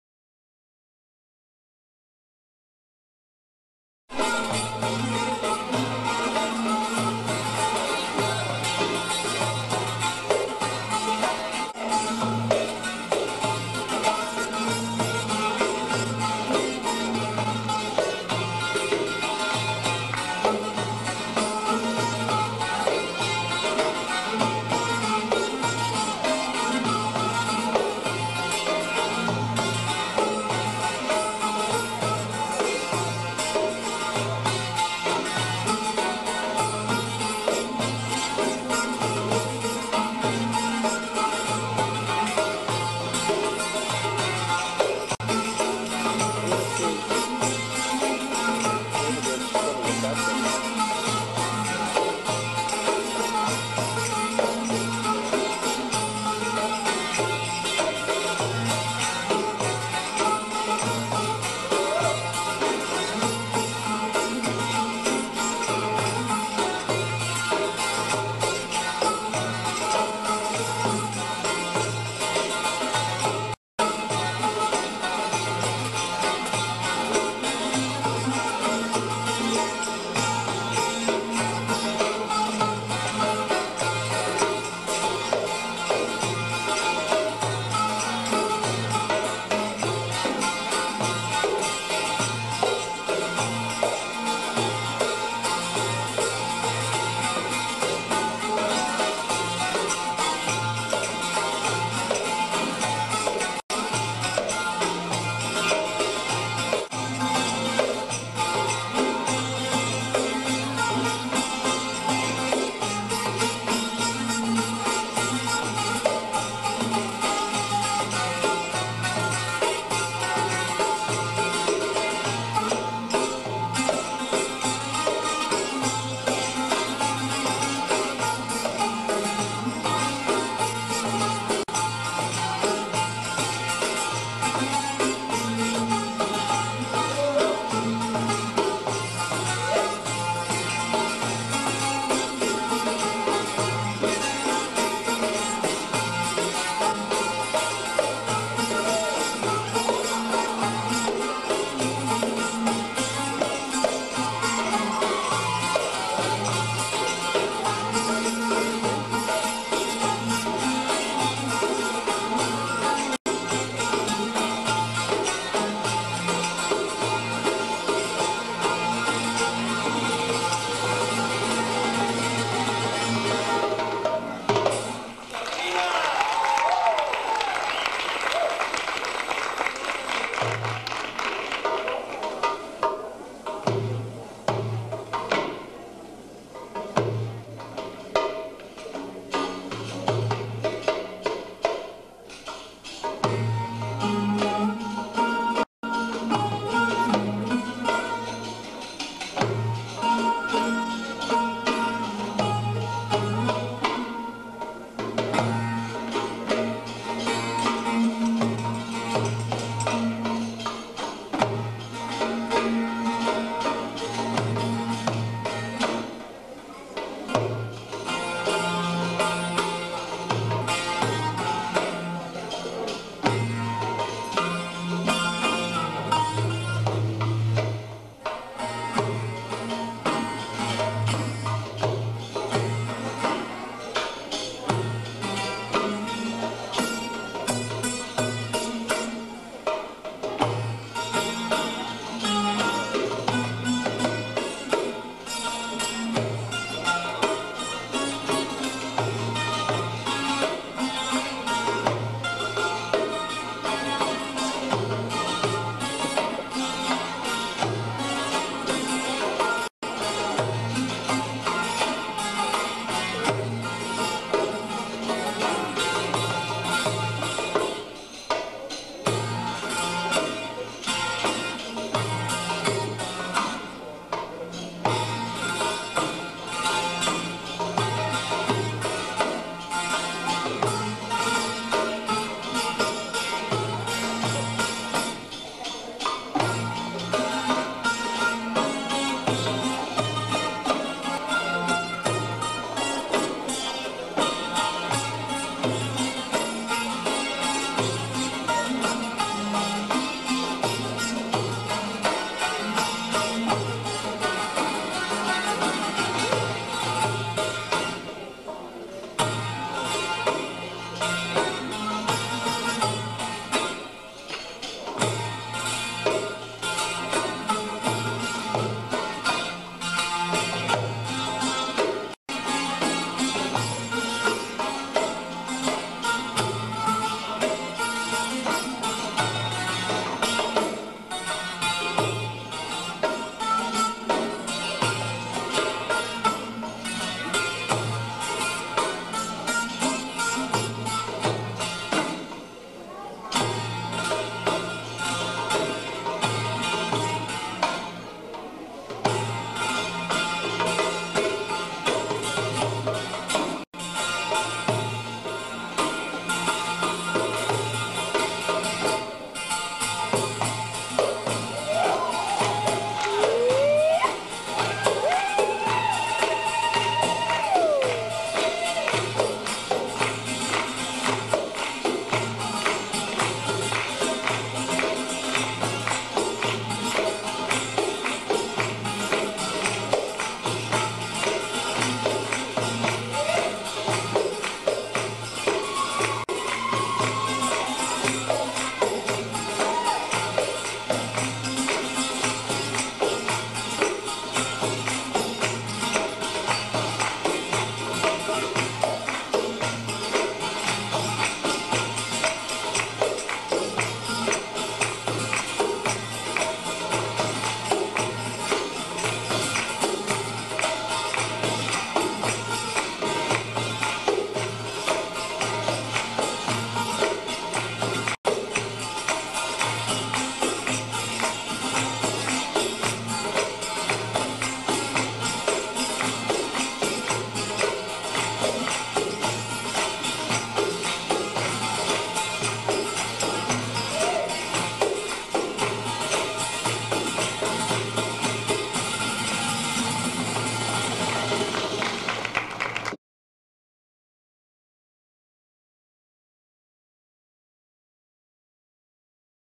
"Sultry Sabrina" American Cabaret with LIVE BAND !
"Sabrina's, American Cabaret Free Style Belly Dance with Zills (finger cymbal) David Fieldhouse, on Tabla, Marta Schill (author of the Complete Belly Dancer instruction on Belly Dance Book) on Tamborine, Jeremy Soto on Def, and Daoud musician on Strings. Mecda Dance, Montclair 1999. Fringe Costume by Sabrina. dedicated to RR of San Diego.